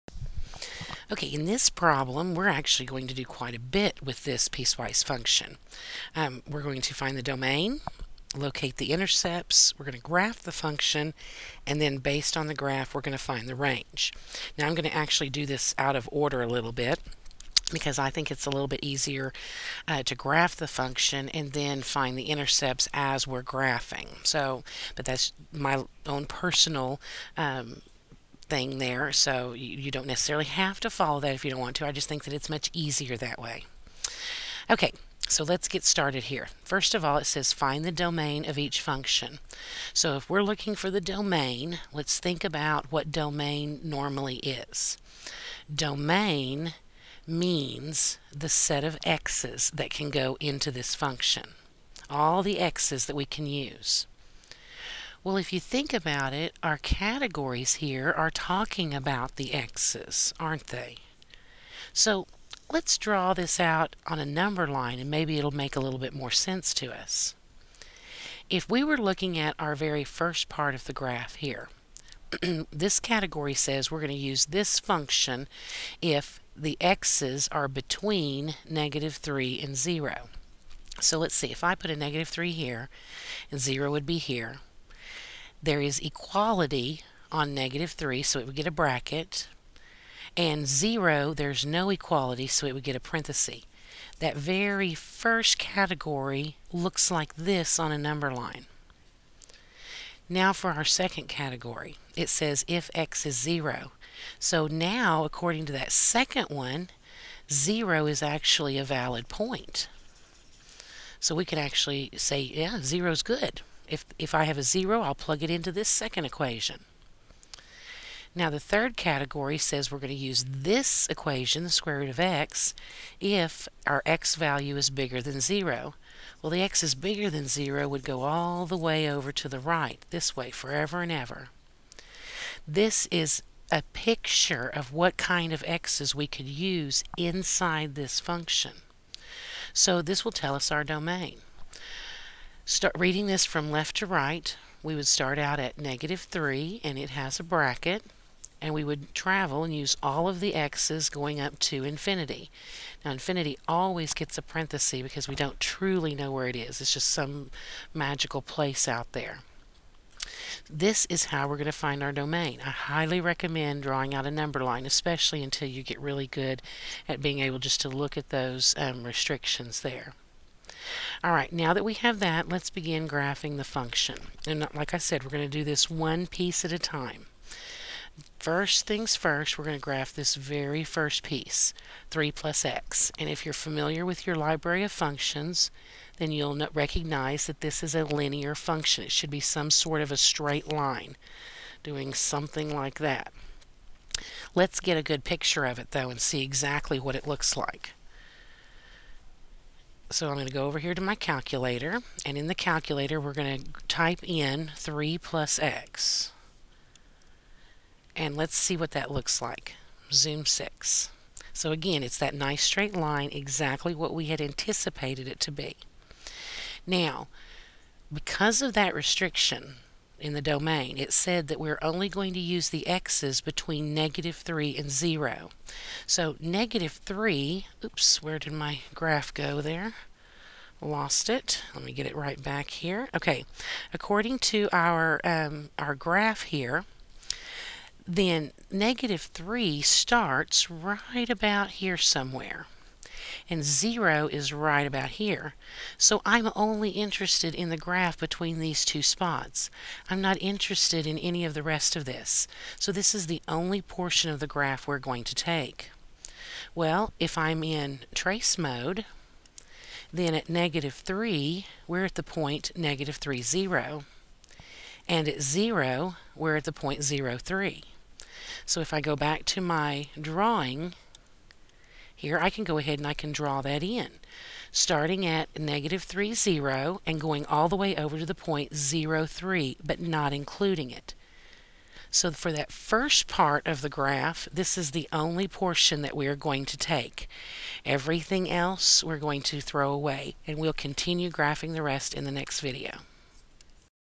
Graphing a Piece-Wise Function part 1 3.4
Finding the domain and beginning to graph a piece-wise